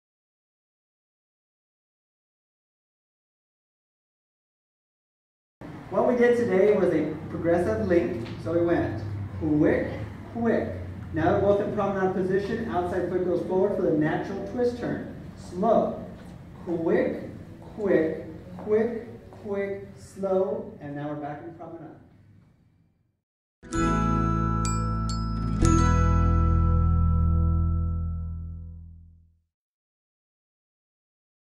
Baile Divino: American Tango (Week 4 of 6) Sept. 28, 2016 [HD]
In this class we learned the:
-Natural Twist Turn